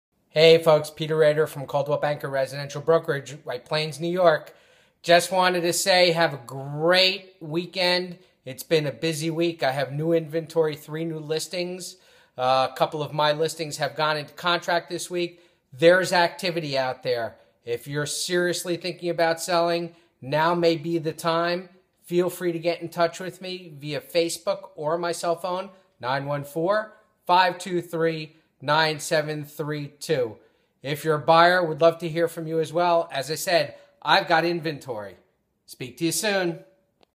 Peter Raider Real Estate Market 2014
Peter Raider has new inventory coming on the market in November 2014 and has a database of buyers who are ready to buy.